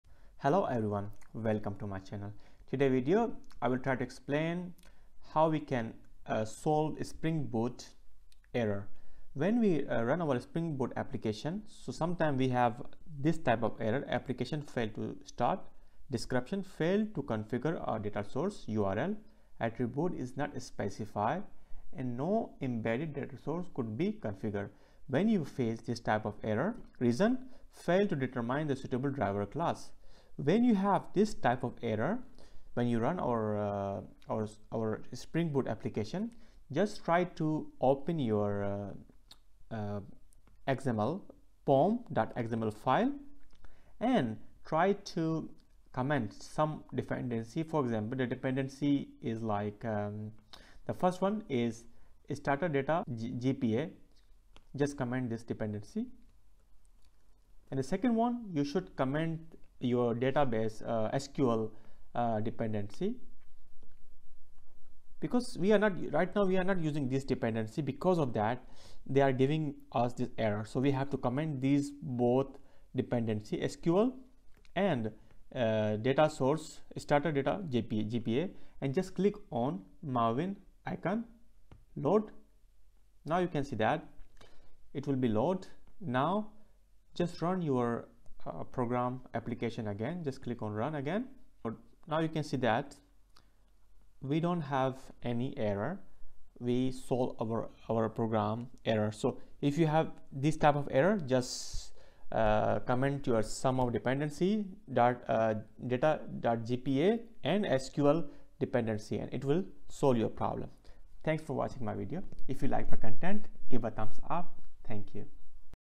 Failed to configure a DataSource: 'url' attribute is not specified and no embedded datasource.
Hello, welcome to the KhanPedia (KP) YouTube channel.

APPLICATION FAILED TO START
Description:

Failed to configure a DataSource: 'url' attribute is not specified and no embedded datasource could be configured.

Reason: Failed to determine a suitable driver class

Action:

Consider the following:
 If you want an embedded database (H2, HSQL or Derby), please put it on the classpath.
 If you have database settings to be loaded from a particular profile, you may need to activate it (no profiles are currently active).

Thanks